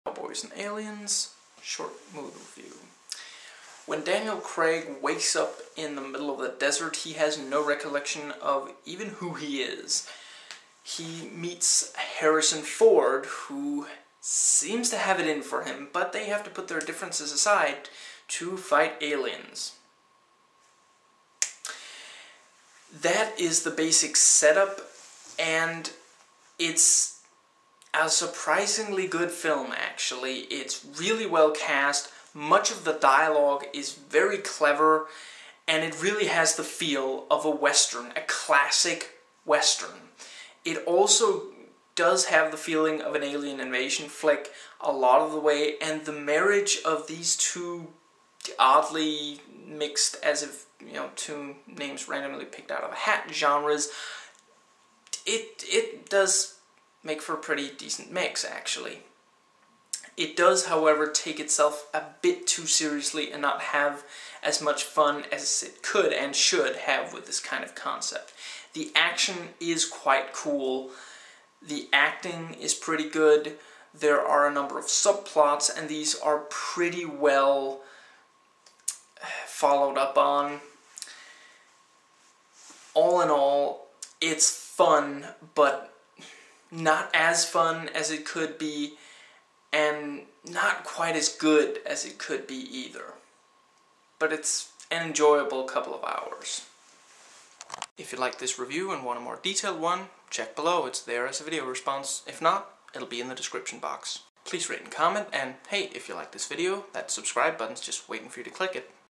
Cowboys & Aliens short movie review
I recorded this yesterday after having watched the movie. I hope you enjoy it, and that I can help spark debate about the film.

The spoiler-ridden(3 and a half minute) video where I go over thoughts I had on the movie